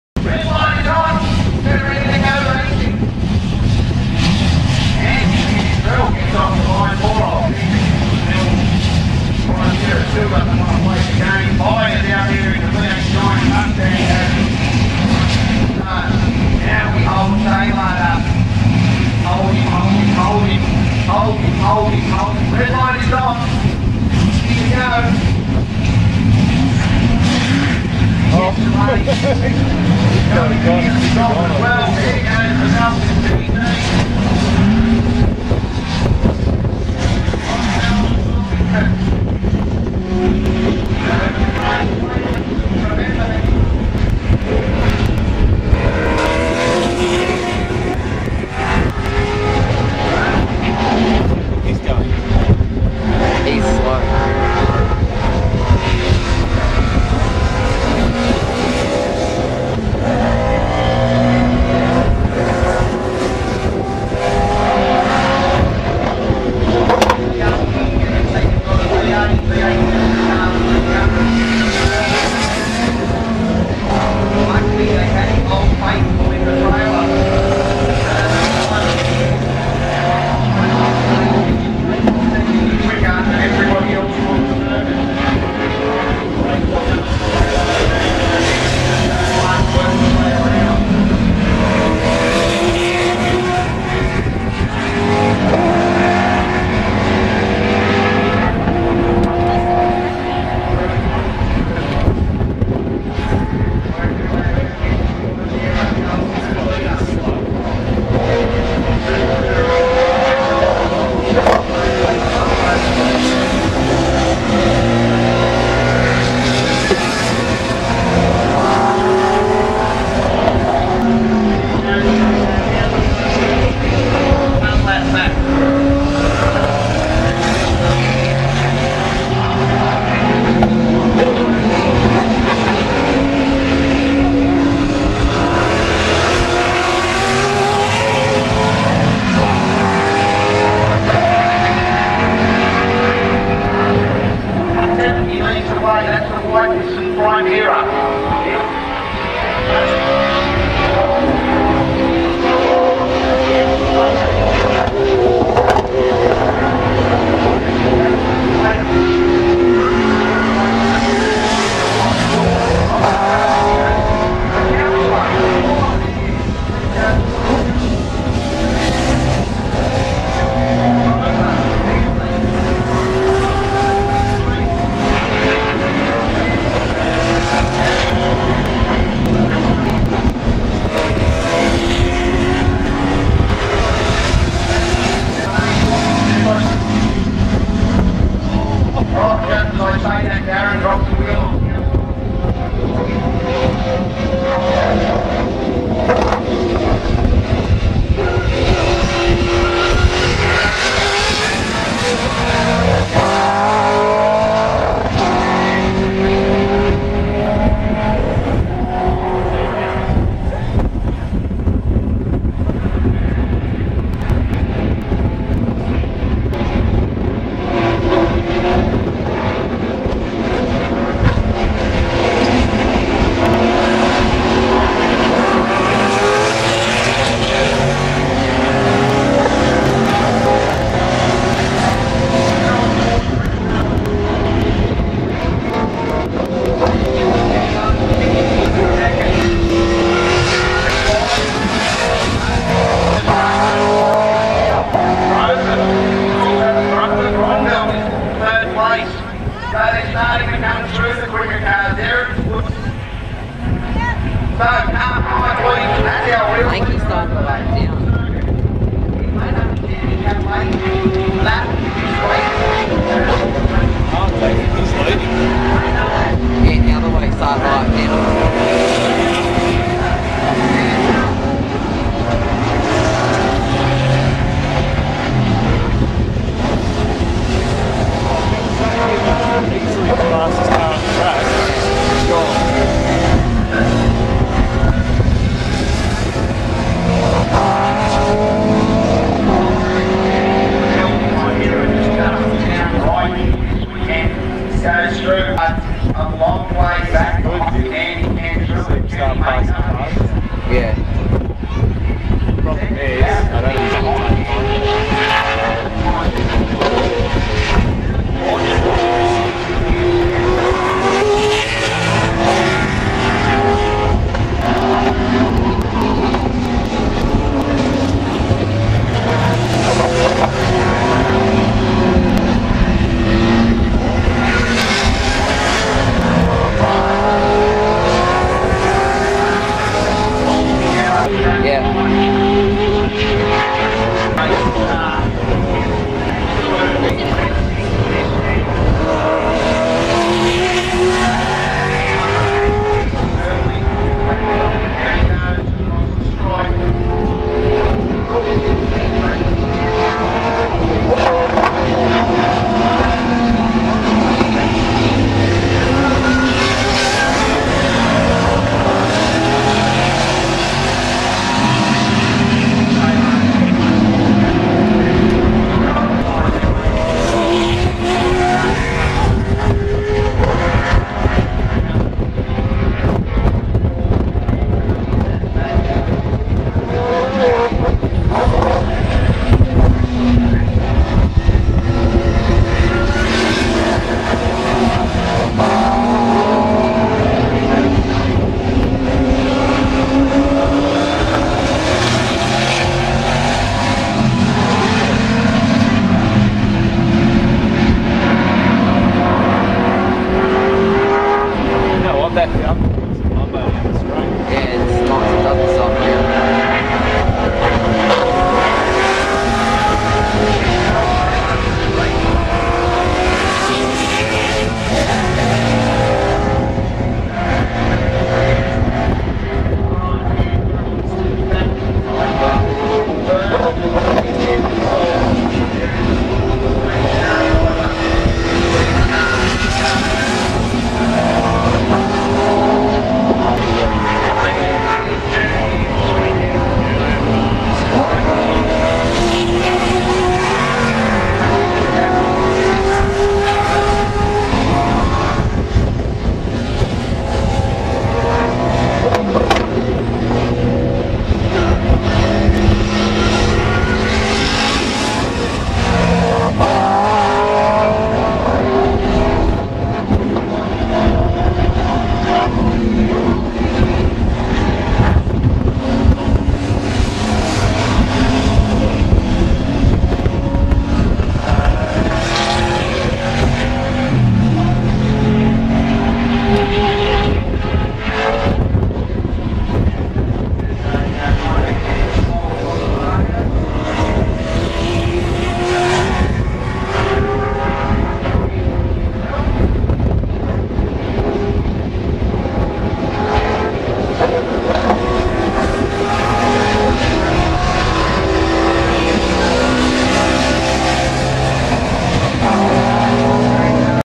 Touring Car challenge Oran Park
The touring car challenge at Oran park raceway 2009. This includes Ex-V8 supercars!

 Very windy day = poor camera work.